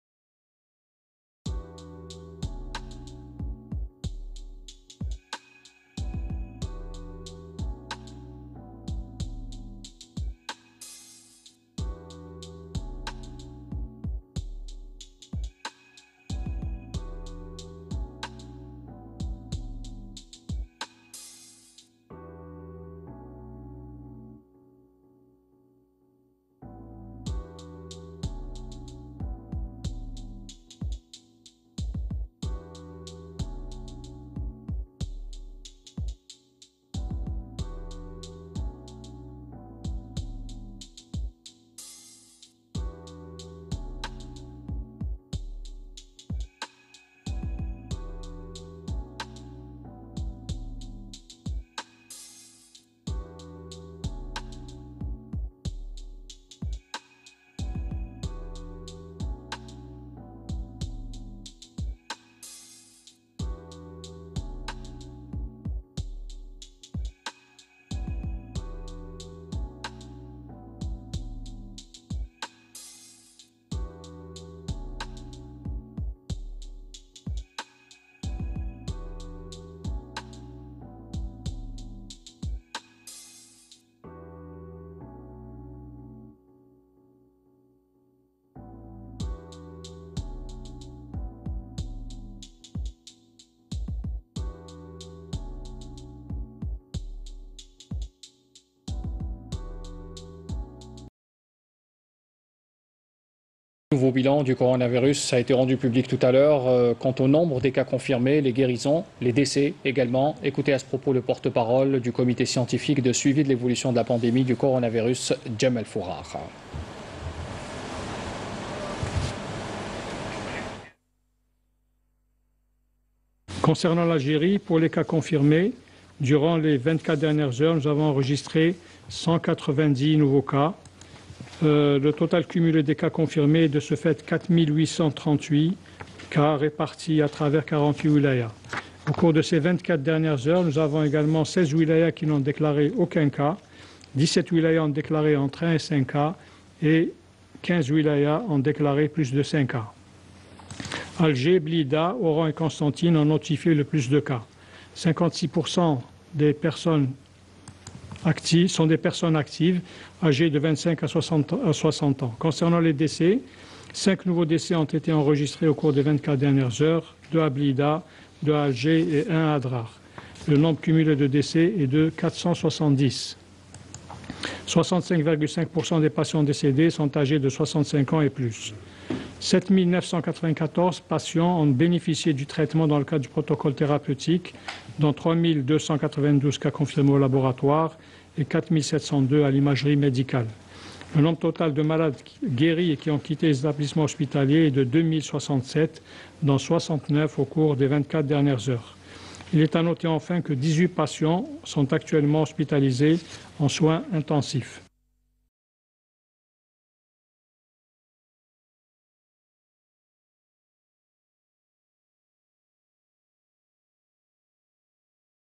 CORONAVIRUSALGERIEBilan duMardi5 Mai 2020
05 nouveaux décès et 69 cas guéris au bilan quotidien du Covid-19 présenté ce mardi par la commission scientifique de suivi de l’épidémie.
Ainsi, le nombre total des décès du Covid-19 en Algérie passe à 470 morts, alors que les nombre des cas de guérison a atteint à ce mardi 05 mai les 2067 cas, selon le dernier bilan présenté par Djamel Fourar porte-parole de la commission.